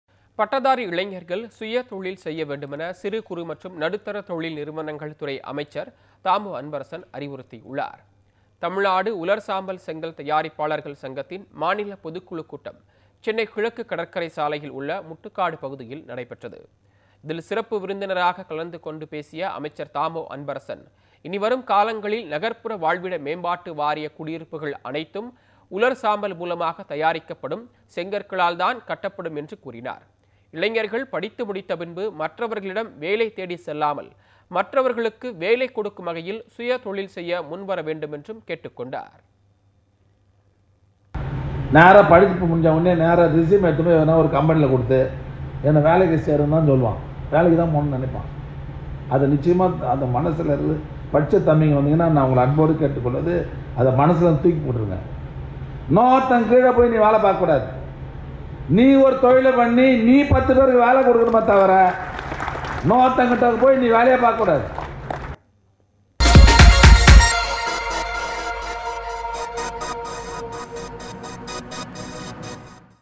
பட்டதாரி இளைஞர்கள் சுயதொழில் தொடங்க முன் வரவேண்டும் - அமைச்சர் தா.மோ.அன்பரசன் பேச்சு
பட்டதாரி இளைஞர்கள் சுயதொழில் தொடங்க முன் வரவேண்டும் சிறு, குறு, நடுத்தர தொழில்துறை அமைச்சர் தா.மோ.அன்பரசன் பேச்சு